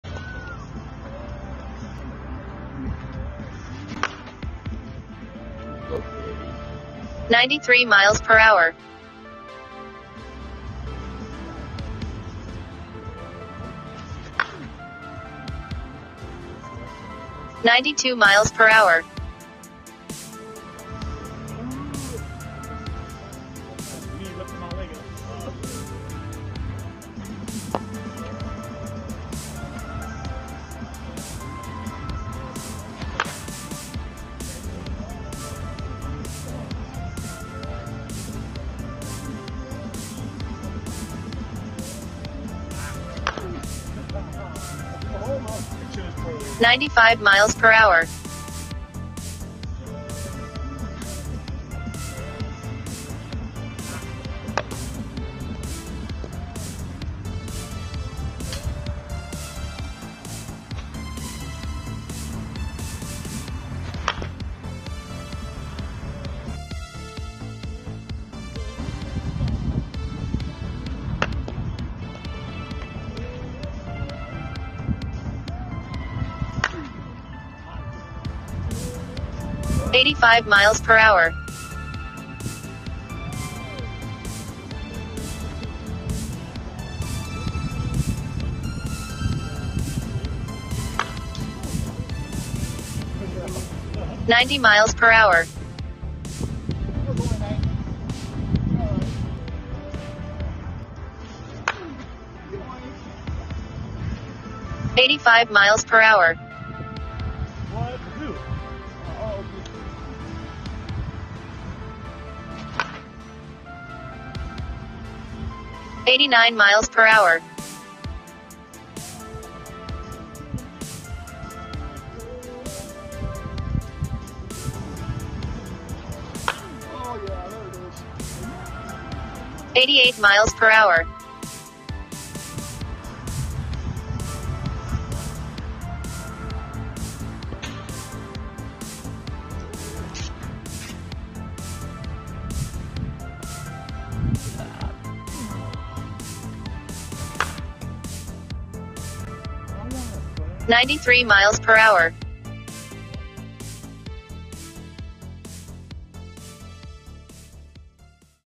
1 hit n Switch Suncoast Melee Reloaded 2 Autism 🧩 Senior bat
last round of BP... 1 hit n switch.. 44/375lbs balls used and 300ft fences